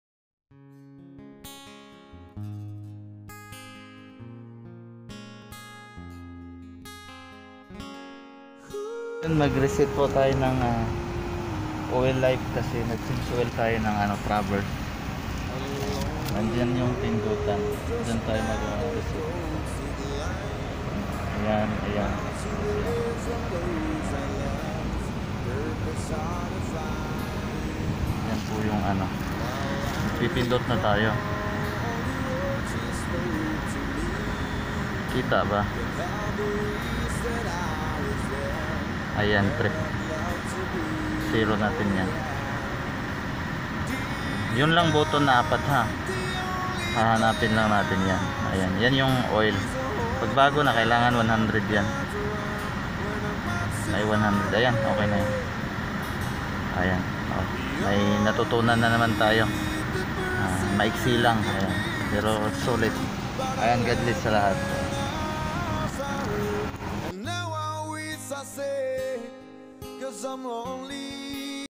Oil Life Reset Chevrolet Maintenance
How to reset oil life after change the engine oil. How to reset maintainance indicator light. Step by step tutorial fir reset oil life.